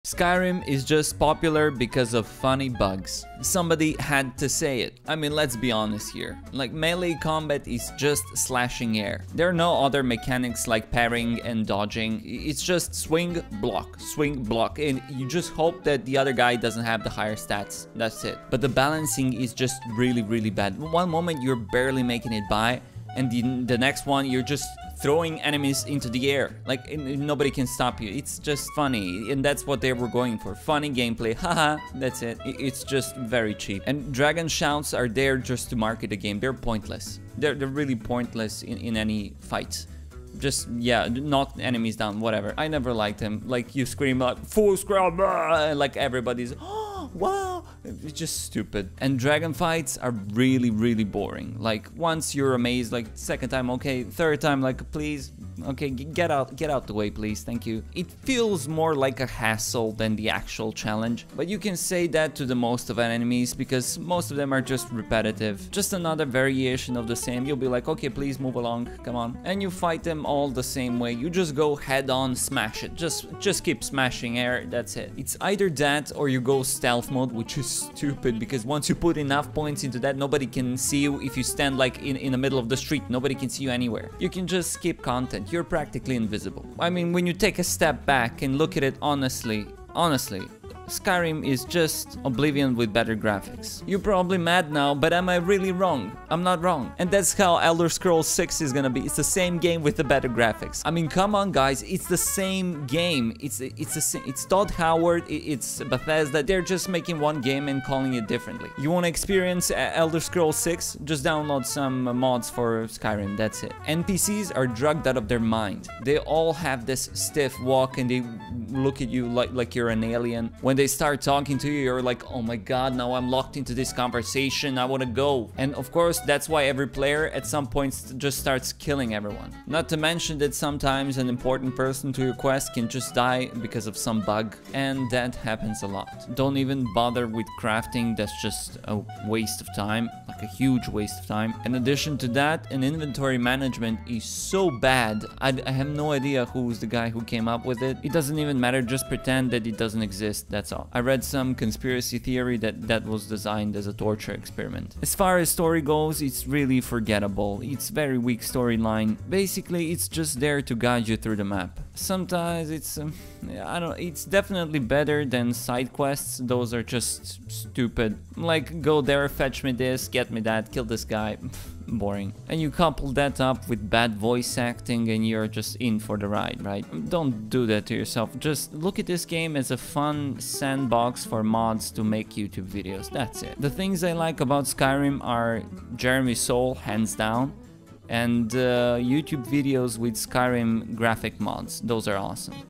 Why Skyrim SUCKS
Let's be honest here, it was only hype. This game was never good. Poor combat, poor NPC-s, poor story. Some places on the map look nostalgic and coupled with Jeremy Soule that really brings out the feeling. But realistically this game is just a one big meme. From the very beginning there were thousands of bugs. Not to mention that hey released the same game every year and somehow it gets worse.

0:00 Why is it popular?
0:04 Combat
0:18 Balance
0:35 Shout
0:49 Dragon fights
1:02 Enemy Variation
1:15 Stealth
1:26 Same as oblivion
1:48 How to play elder scrolls 6
1:53 NPC-s
2:09 Bug
2:16 Crafting
2:21 Inventory
2:35 Story
2:42 Quests
2:52 Voice Acting
3:03 PROS